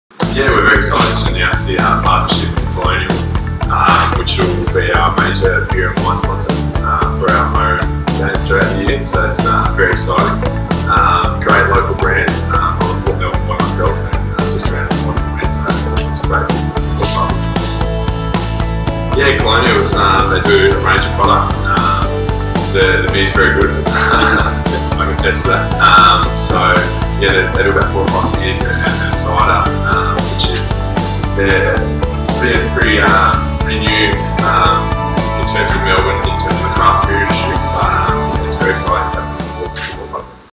Colonial Brewing Co: Official Sponsor of Essendon AFL
AFL team Essendon announced Colonial Brewing Co as their official beer and wine sponsor for their home games throughout the year.

Exclusively for Bohemian Rhapsody Magazine bohemianrhapsodyweekly.weebly.com